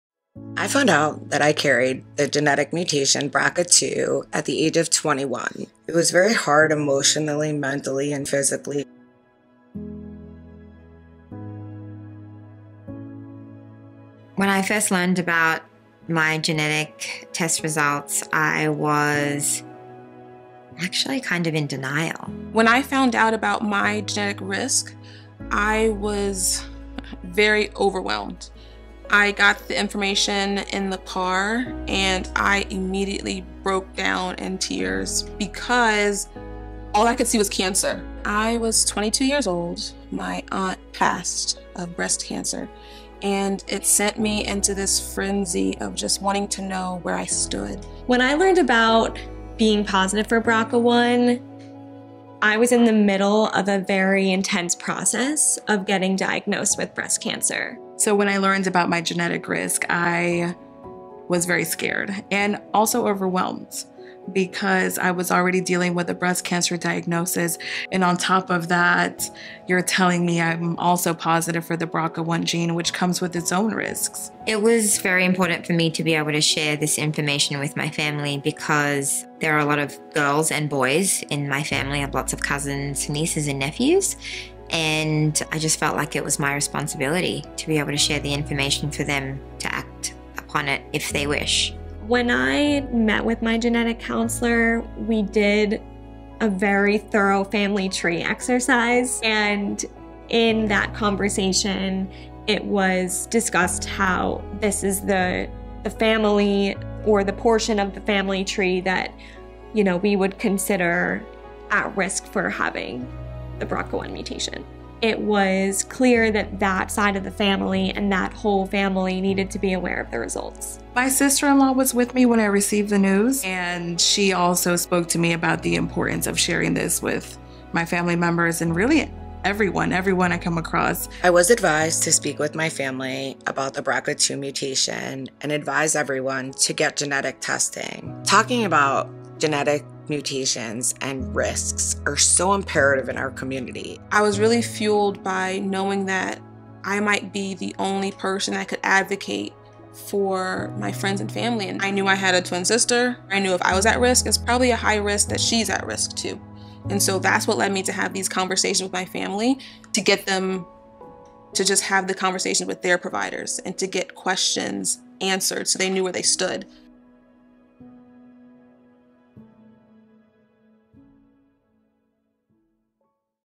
The Importance of Sharing Your BRCA Genetic Test Results with Family
Hear how women who have tested positive for harmful changes in their BRCA genes have dealt with being the first in their families to learn about their high risk for breast cancer. Watch as they talk about the importance of sharing their genetic test results with their family members whose health may also be affected by this information.